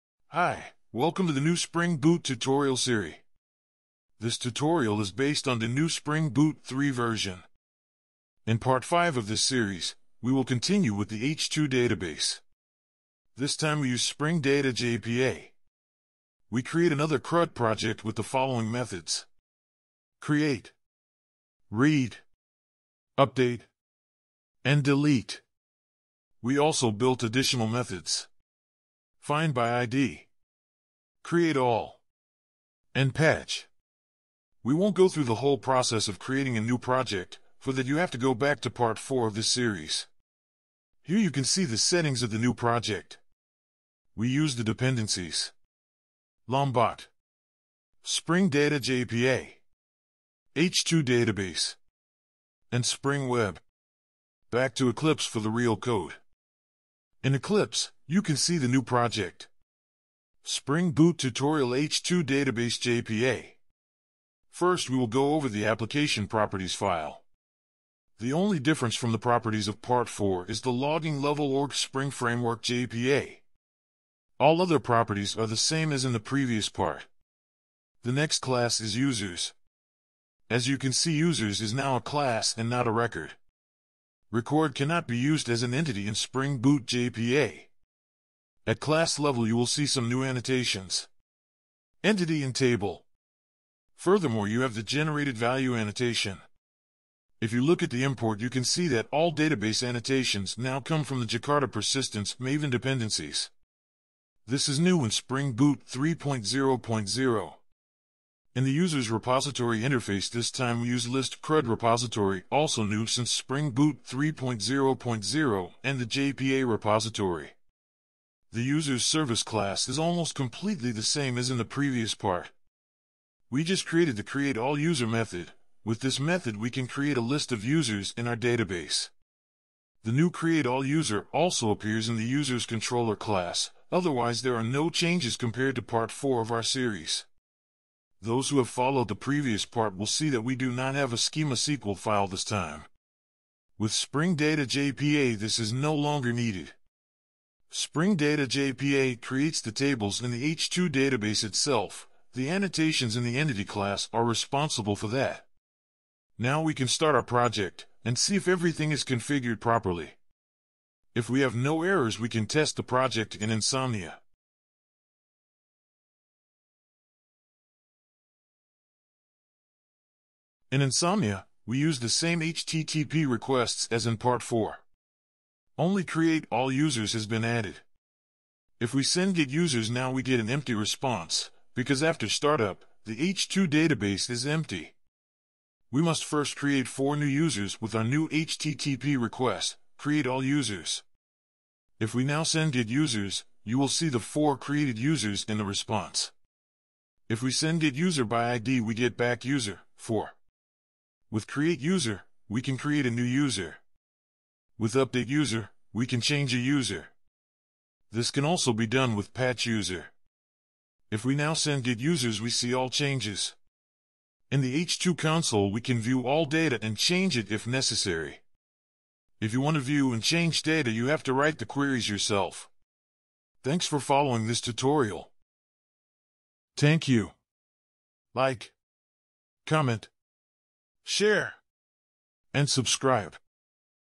Spring Boot 3.0.0 tutorial (part 05) - Spring Data JPA & H2 Database
Spring Data JPA
H2 Database
Insomnia
GitHub